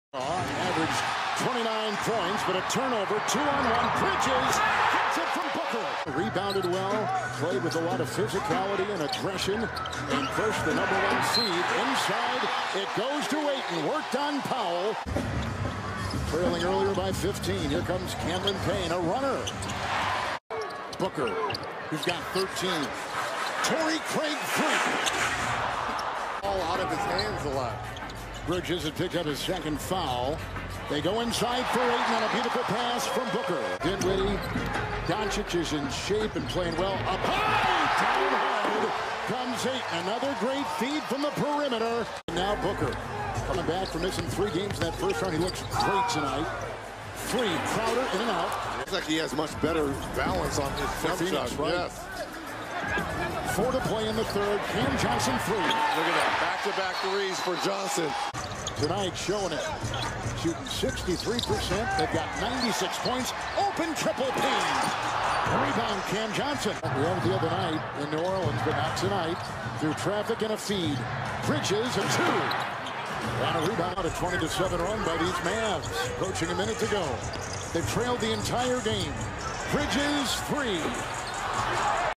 Devin Booker Passing Vs Mavericks (2022 Playoffs Semifinals Game 1)
Underrated playmaker when his teammates function properly

Follow @RyB_311 on Twitter